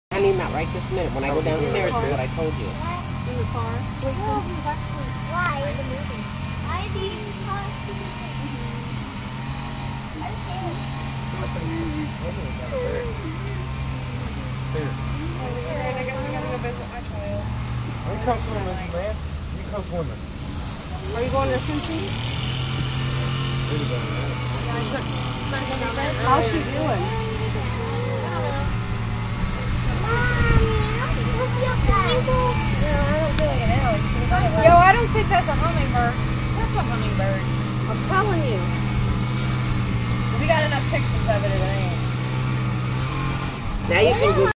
Video of a hummingbird Moth
This is a hummingbird Moth getting nectar from our petunias. We thought it was a little hummingbird at first! LOL